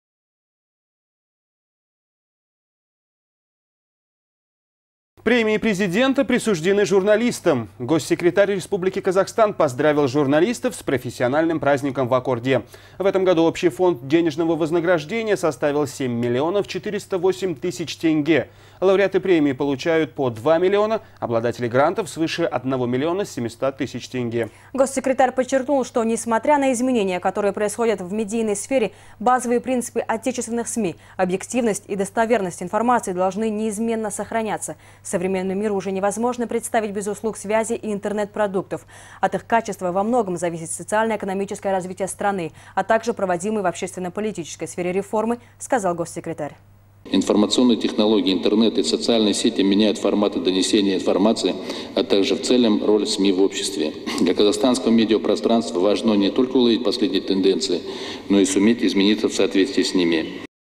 Премии Президента присуждены журналистам
Госсекретать РК поздравил журналистов с профессиональным праздником в Ак орде. В этом году общий фонд денежного вознаграждения составил 7 миллионов 408 тысяч тенге. Лауреаты премии получают по 2 миллиона, обладатели грантов - свыше 1 миллиона 700 тысяч тенге.